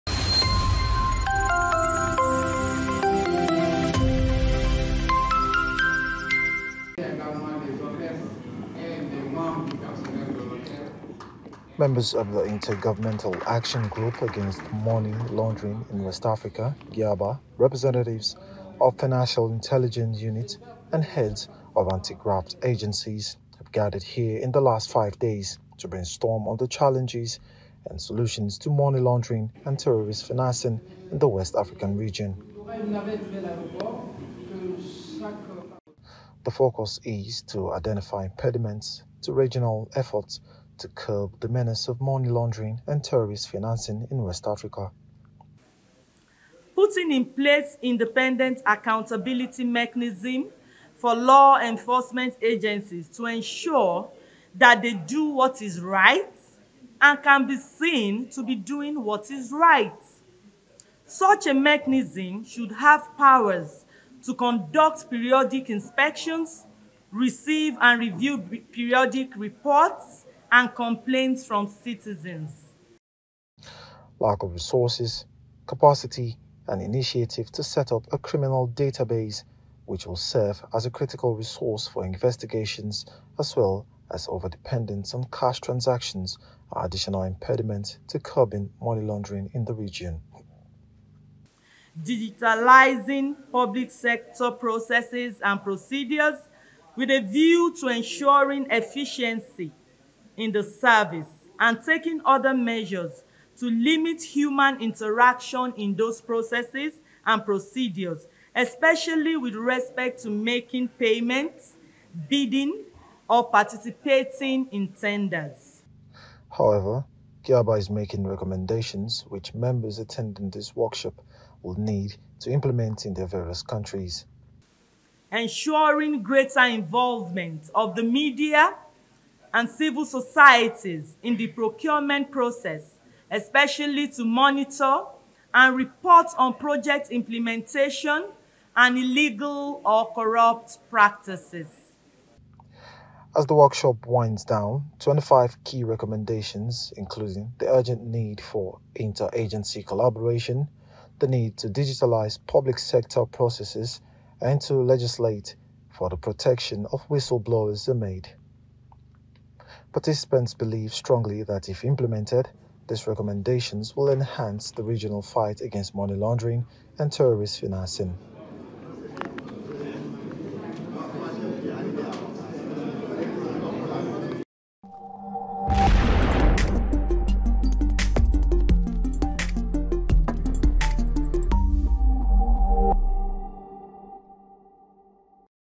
TACKLING MONEY LAUNDERING: ECOWAS AGENCY RECOMMENDS STRONGER REGIONAL COLLABORATION AMONG MEMBERS
controltvng 2023CTVerdict TACKLING MONEY LAUNDERING: ECOWAS AGENCY RECOMMENDS STRONGER REGIONAL COLLABORATION AMONG MEMBERS